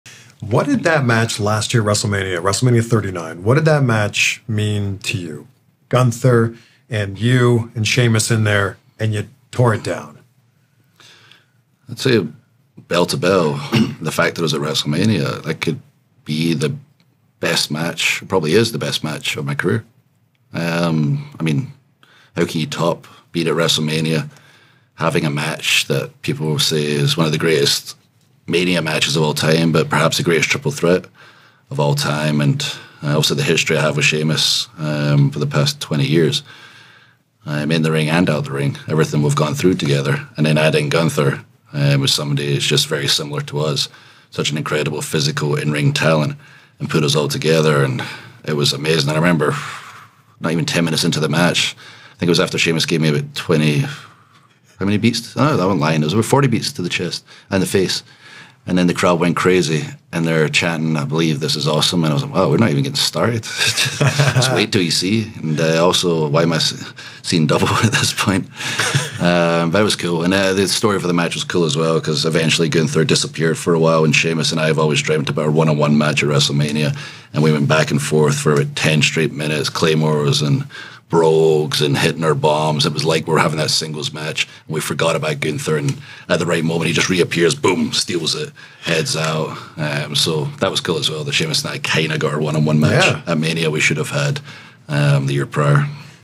Drew McIntyre vs Gunther vs Sheamus at Wrestlemania
WWE Superstar Drew McIntyre joins Chris to discuss his Wrestlemania 39 match against Gunther and Sheamus.